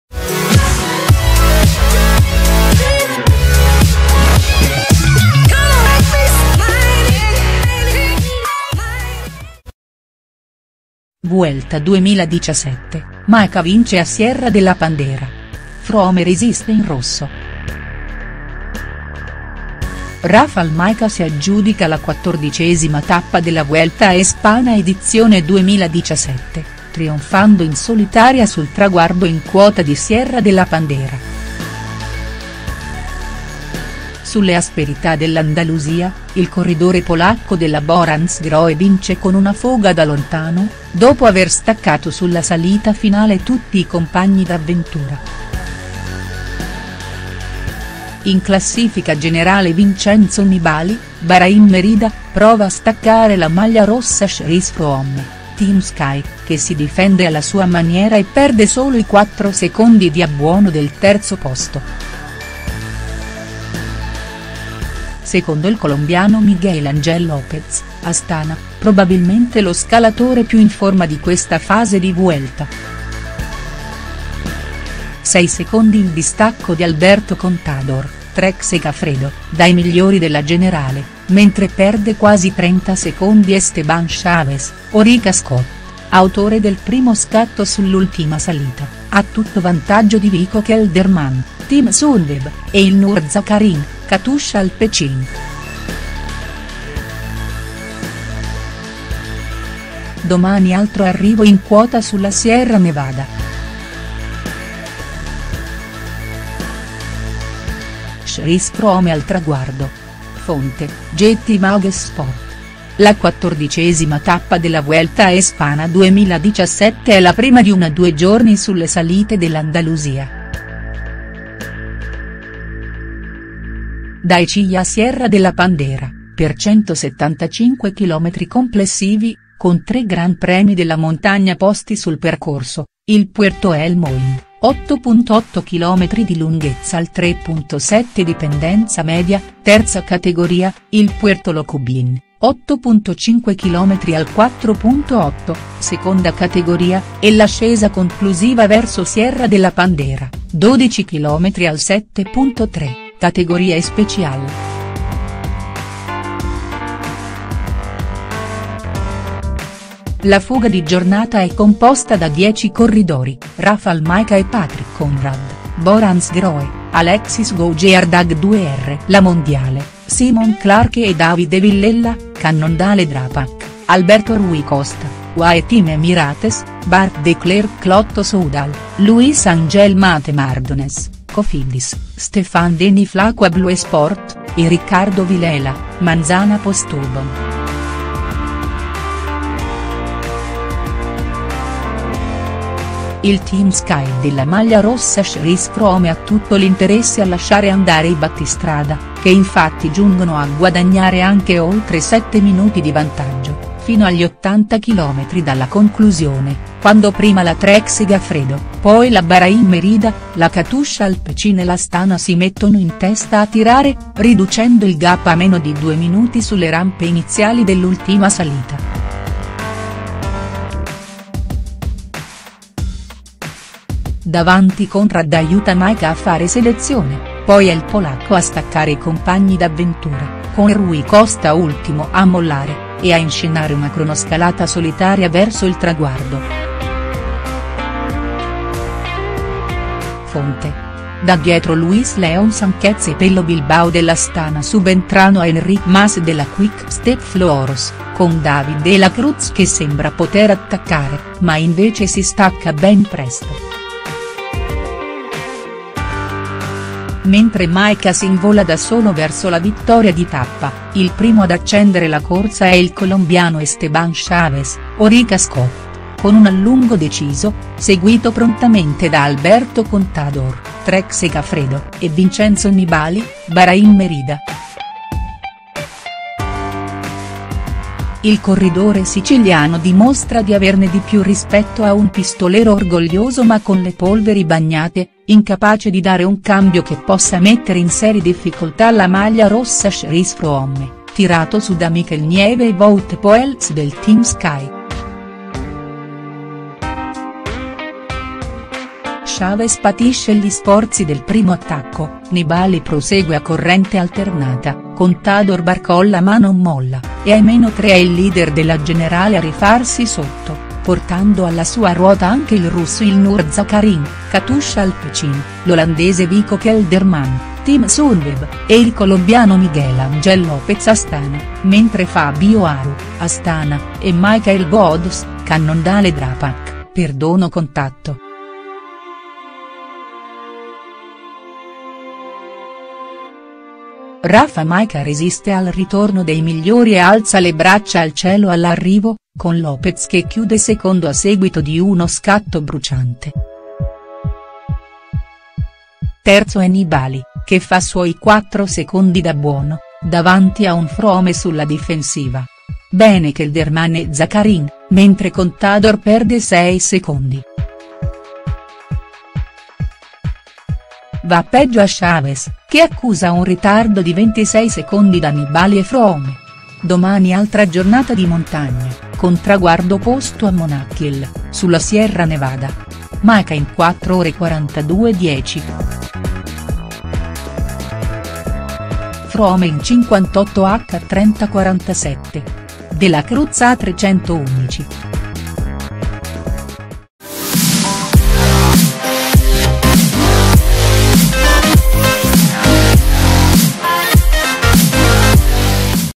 Vuelta 2017, Majka vince a Sierra de La Pandera. Froome resiste in rosso.HD
Il polacco vola in salita al termine di una lunga fuga. Nibali attacca Froome, ma guadagna solo quattro secondi di abbuono. Chaves in difficoltà, Contador perde sei secondi.